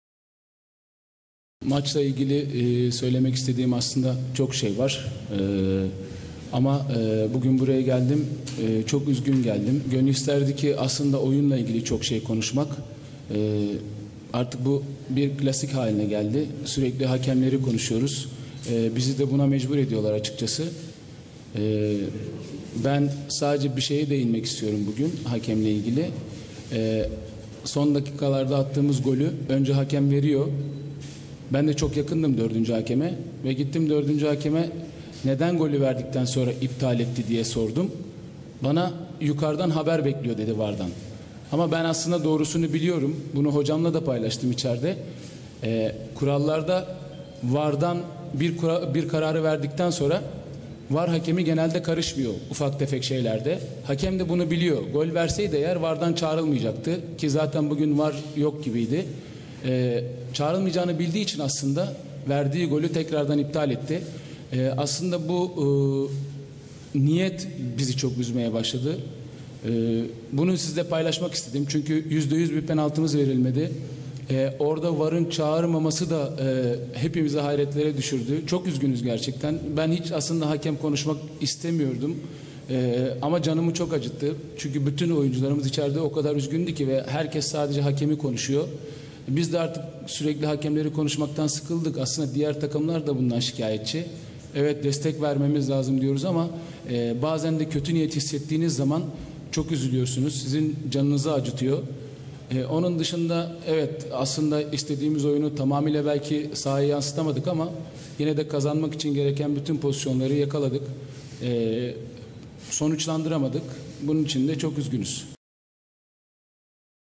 Selçuk İnan: "Hakemlerle İlgili Büyük Sıkıntımız Var!" (Yeni Malatyaspor 0-0 Galatasaray)
Selçuk İnan: "Hakemlerle İlgili Büyük Sıkıntımız Var!" (Yeni Malatyaspor 0-0 Galatasaray) / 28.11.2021

Galatasaray'da yardımcı antrenör Selçuk İnan, Malatyaspor maçı sonrası açıklamalarda bulundu.

İŞTE SELÇUK İNAN'IN SÖZLERİ:

"Mustafa Cengiz bizim için büyük kayıp. Çok üzüldük maçtan önce. Allah rahmet eylesin.

Avrupa maçlarından sonra istediğimiz sonuçları alamıyoruz. Bizi oyundan daha çok üzen dış faktörler oluyor. Hakemlerle ilgili konuşmak istemiyorum ama iyi niyet hissetmediğim için konuşmak istiyorum.

Orta hakem önce golü veriyor. Ardından birden iptal ediyor. 4. hakeme sordum, o bilmiyor ama biz biliyoruz. Bugün VAR yoktu herhalde. Hakem önce golü veriyor ama VAR'ın çağırmadığını bildiği için iptal ediyor. 4. hakeme de söyledim, işgüzarlık yapıyor.

İstediğimiz performansı ortaya koyamadık o bizim sorumluluğumuz ama hakemlerle ilgili de büyük sıkıntımız var. İkinci sarı kart pozisyonu var, penaltı pozisyonunda Emre ayağına basamadı.

Çok pozisyona girdik ama kazanamadık. Üzgünüz."